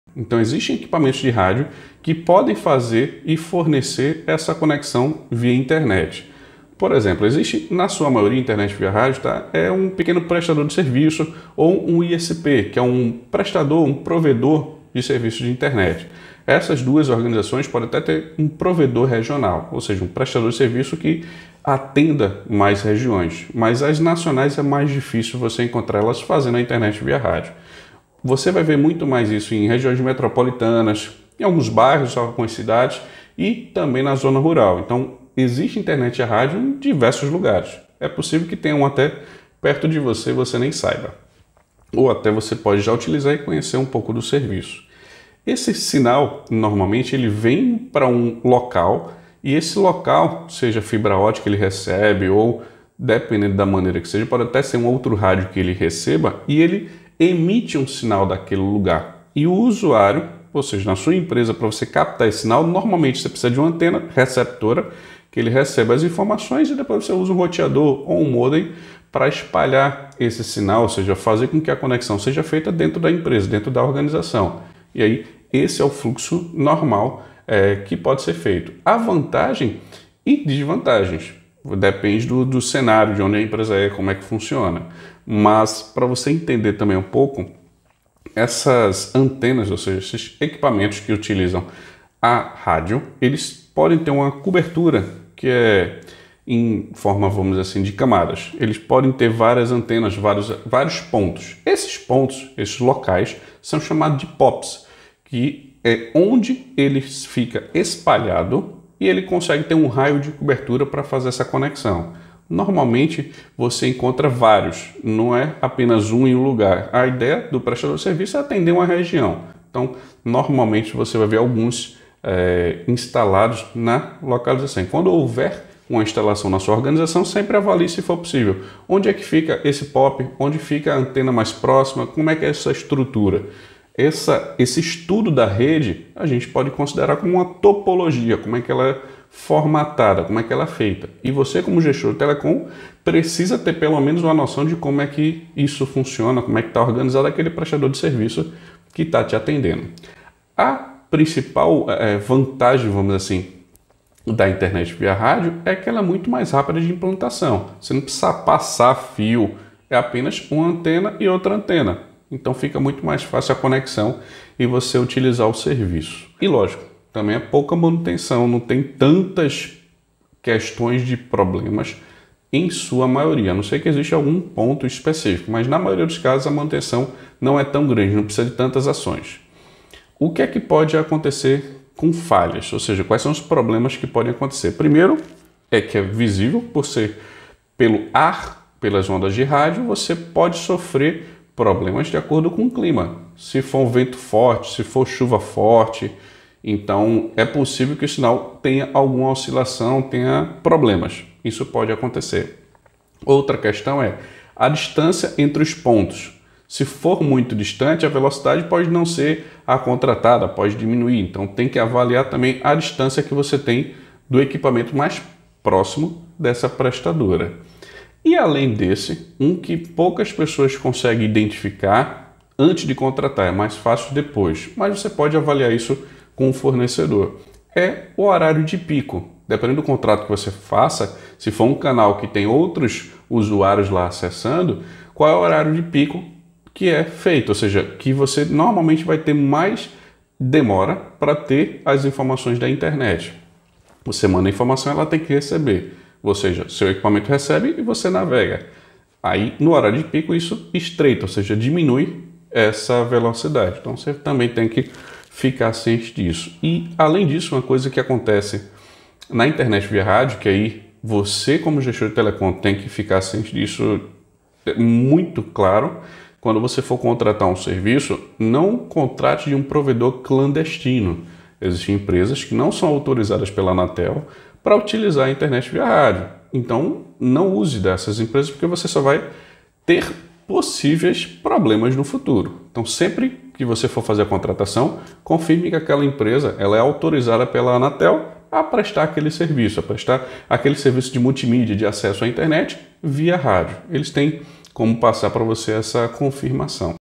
USANDO SERVIÇOS VIA RÁDIO PARA TER ACESSO A INTERNET POR UM PRESTADOR DE TELECOM
Neste vídeo te mostro um pouco do funcionamento da contratação de internet a rádio para uma empresa, mostrando essa possibilidade e um pouco de como ela é.

Dúvidas?
Manda um comentário...

Até+